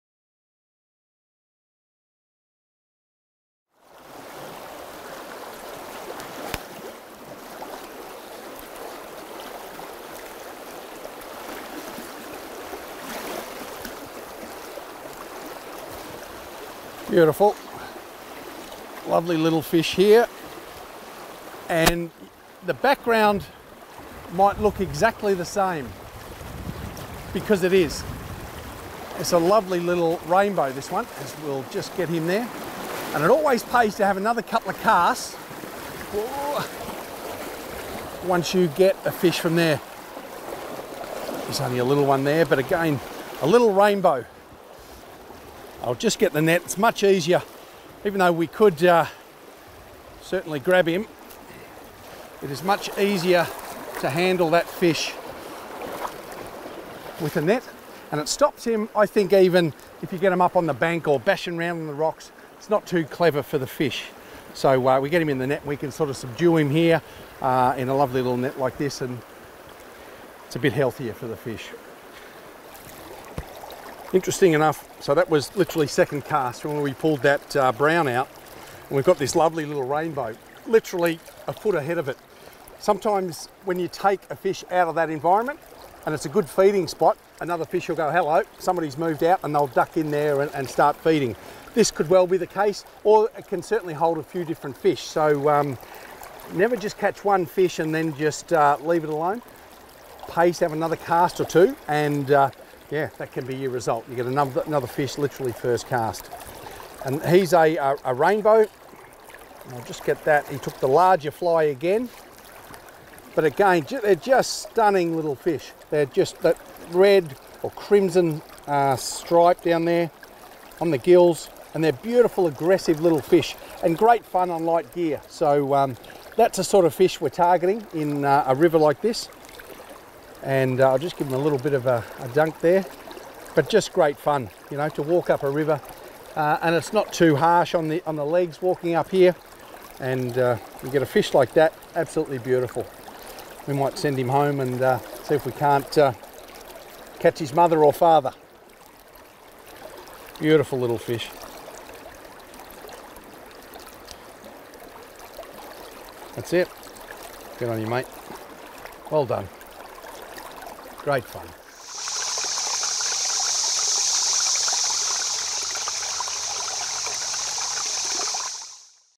Gavin Hurley fly fishing in Victoria's small waters 5
Gavin Hurley tackles some awesome fish on the fly